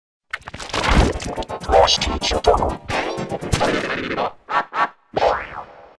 Klasky Csupo in G-Major 32 (VEGAS Pro 20 version)
I used some substitute plugins because I didn't have enough requirement plugins for that effect.

Credits to Klasky Csupo & TBWVE571.